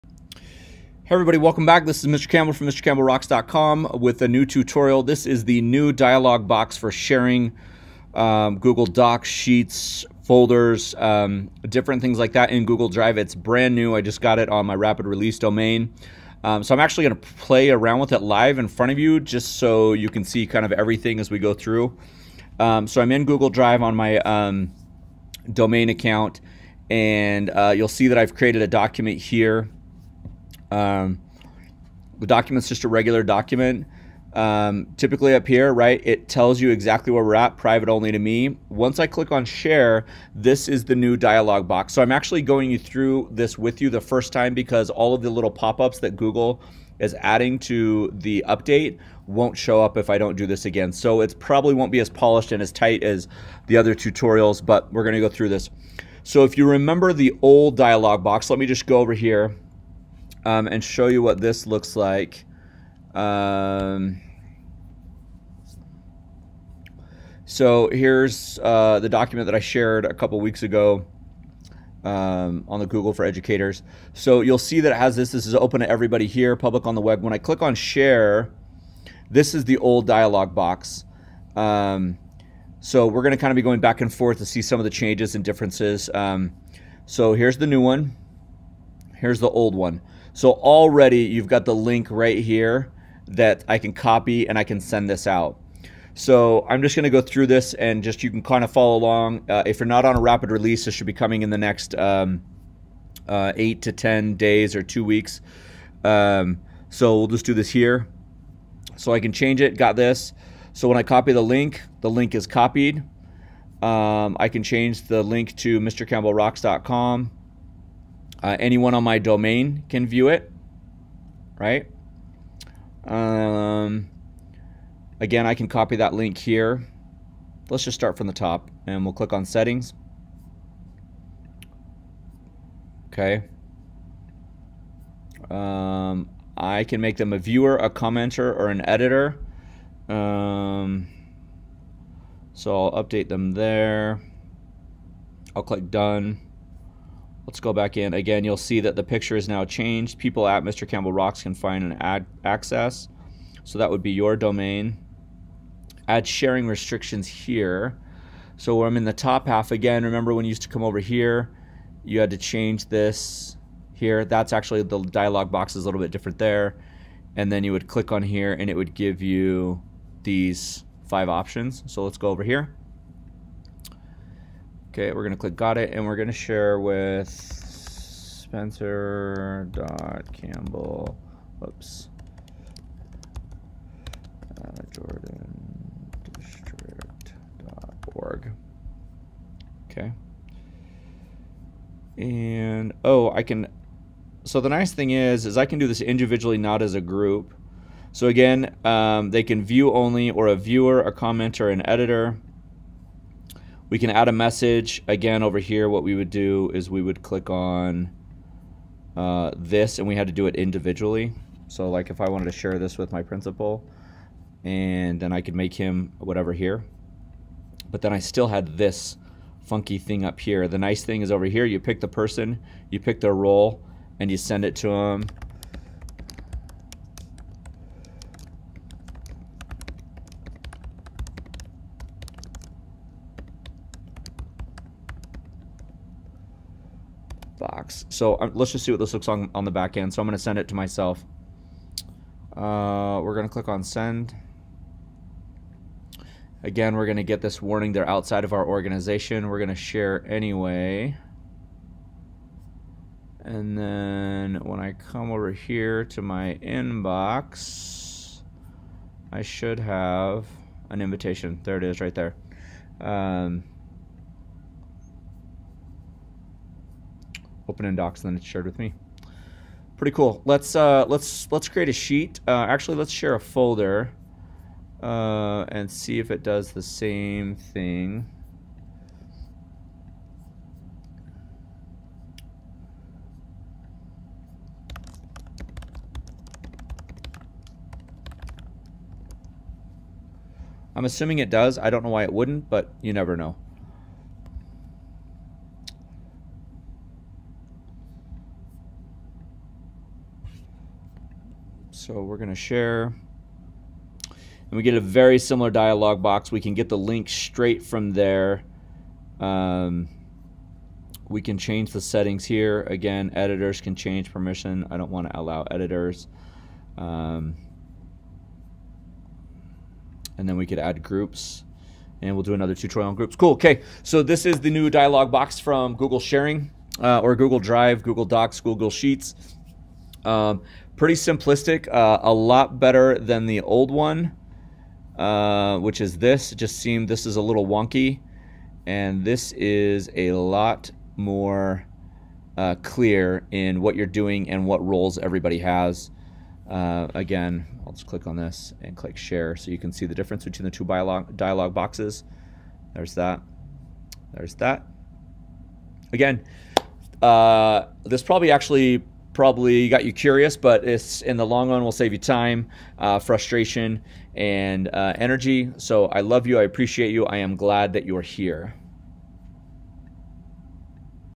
Google Sharing Dialogue Box Update (May 2020 Release)
Google Sharing Dialogue Box (Update May 2020) Google cleaned up their sharing dialogue box when sharing Docs, Sheets, Folders, Forms, and Drive items. It's brighter, less confusing, and gives easier control over people's roles when sharing files. Rapid Release G Suite clients should see it in the next few days and Scheduled Release users should see it in a few weeks.